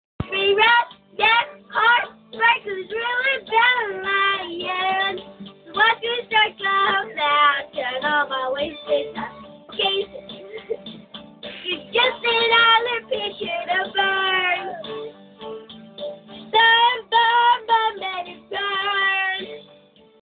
Lexi sings-secret phone video
I secretly took this video of one of my students singing...she thought I was just texting...until I told her at the end of her lesson that I recorded a little of her singing.  Lol.